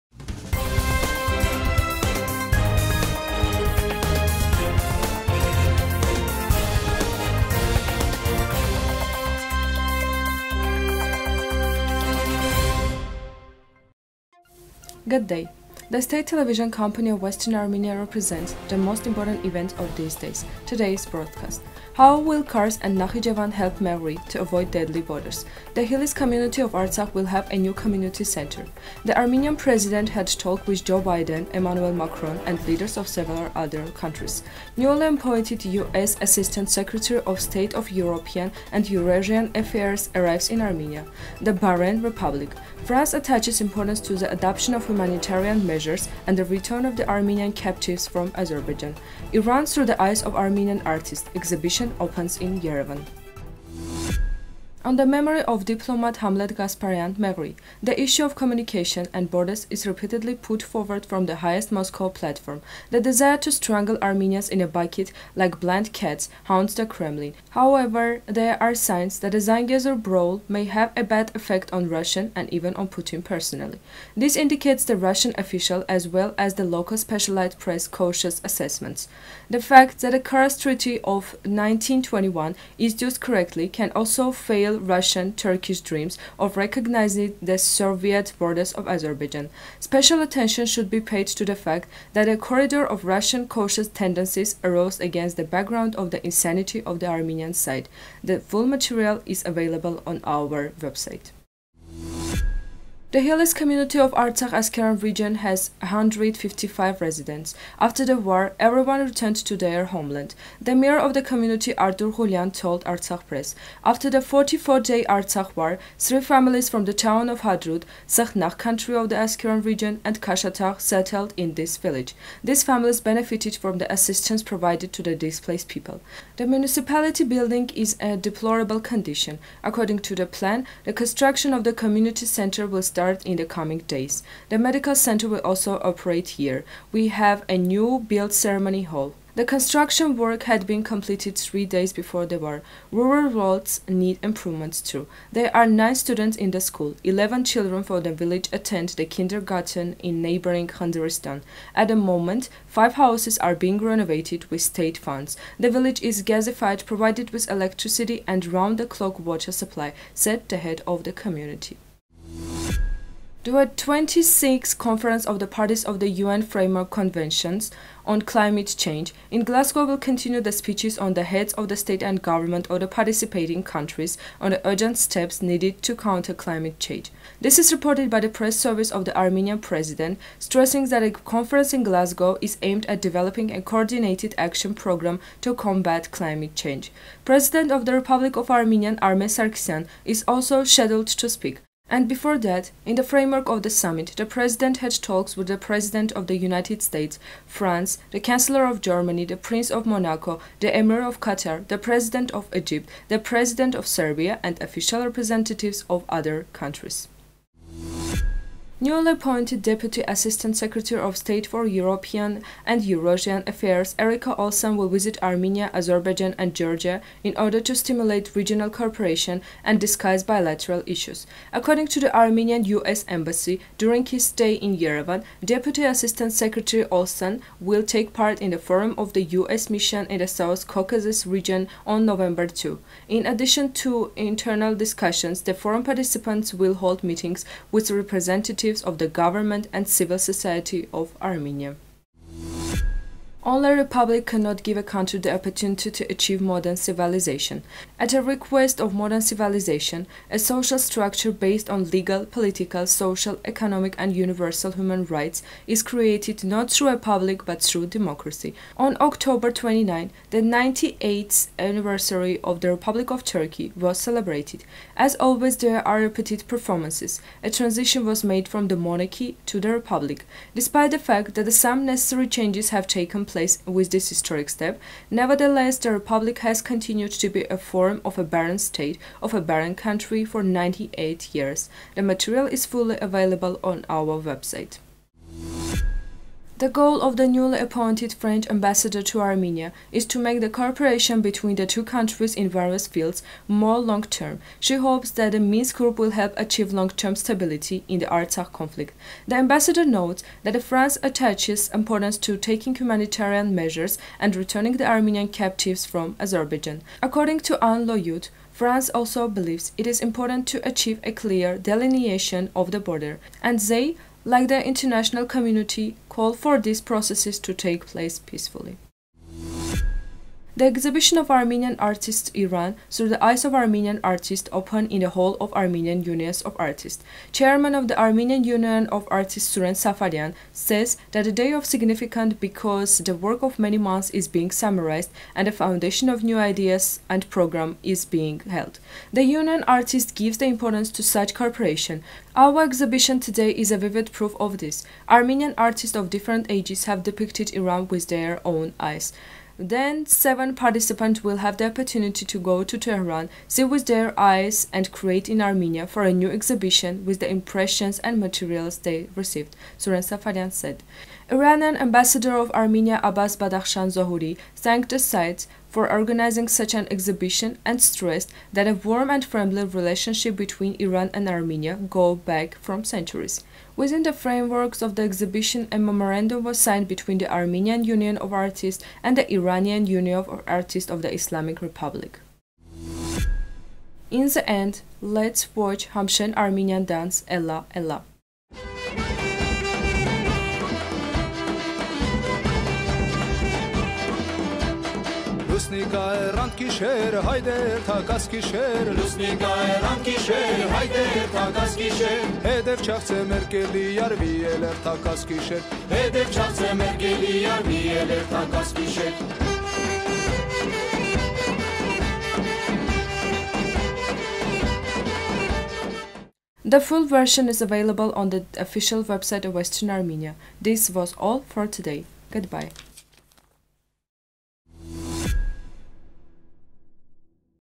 The Barren Republic. 02-11-2021
1.How will Kars and Nakhichevan help Meghri to avoid deadly borders?
2.The Hilis community of Artsakh will have a new community center.
3.Armenian President held talks with Joe Biden, Emmanuel Macron and leaders of several other countries.
4.Newly appointed US Assistant Secretary of State for European and Eurasian Affairs arrives in Armenia.
5.The Barren Republic.
6. France attaches importance to the adoption of humanitarian measures and the return of the Armenian captives from Azerbaijan.
7."Iran through the eyes of Armenian Artists" exhibition opens in Yerevan.